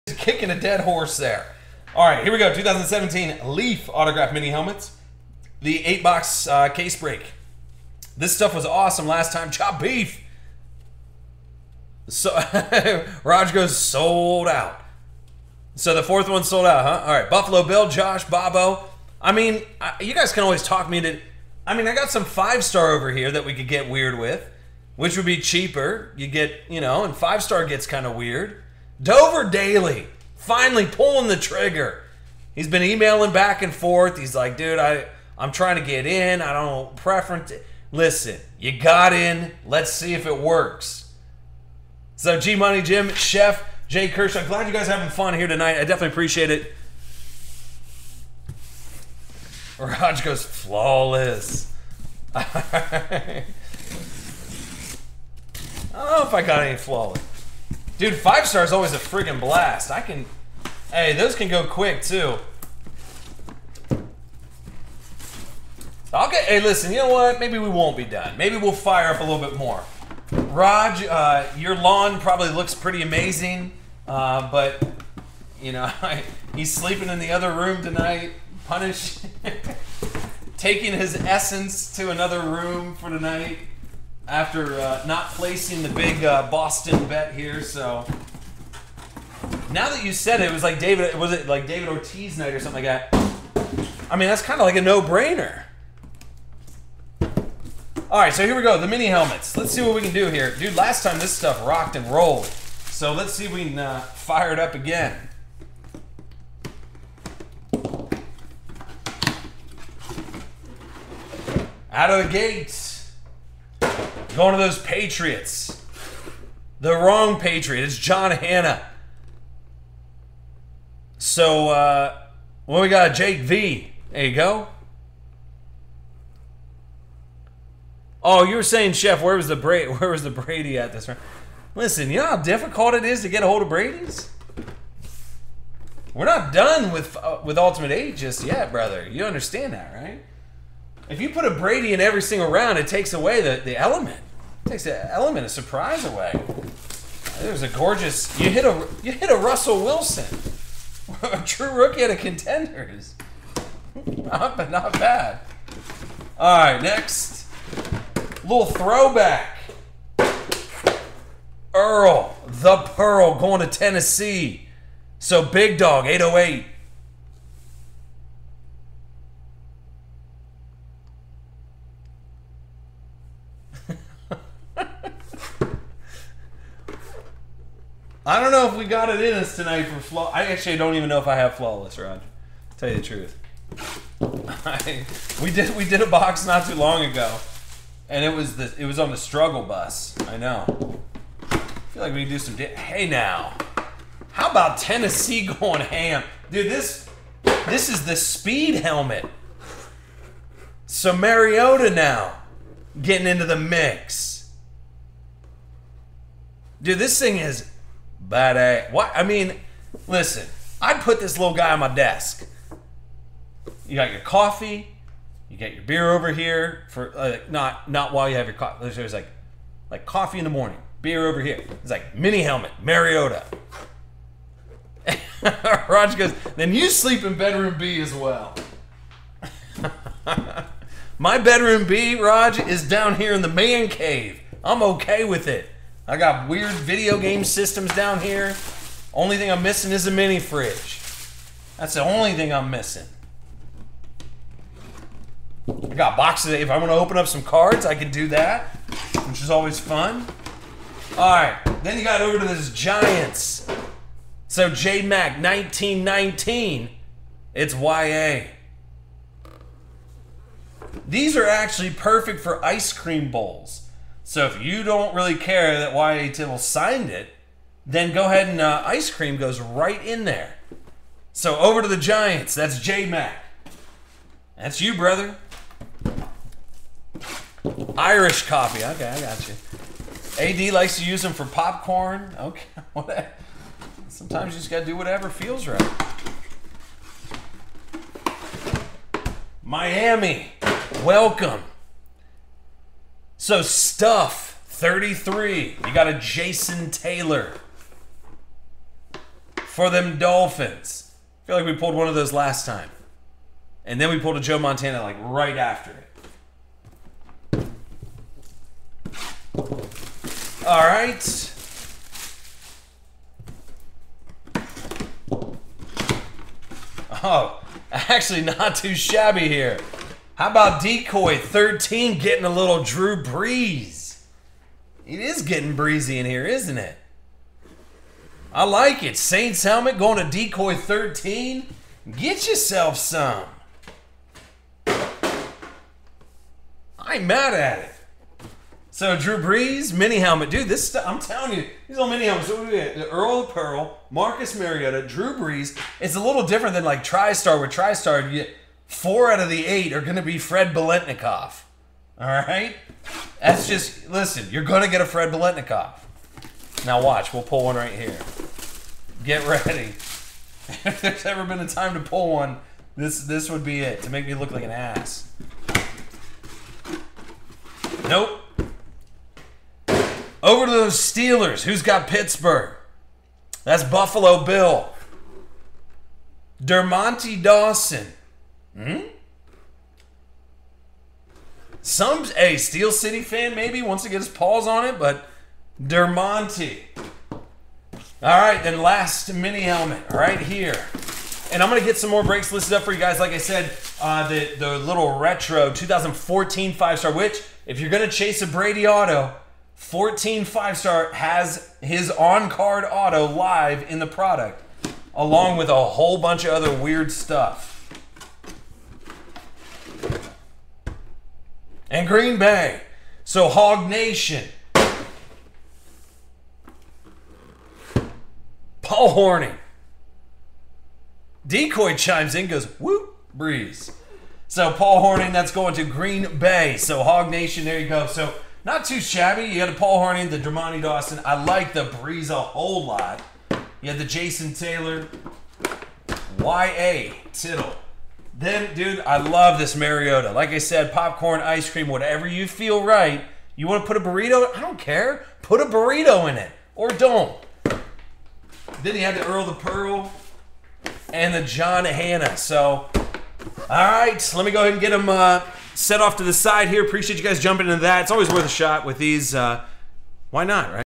UltimateBoxBreaks.com: 2017 Leaf Mini Helmets 8 Box Case Break
UltimateBoxBreaks.com: 2017 Leaf Mini Helmets 8 Box Case Break - Captured Live on Ustream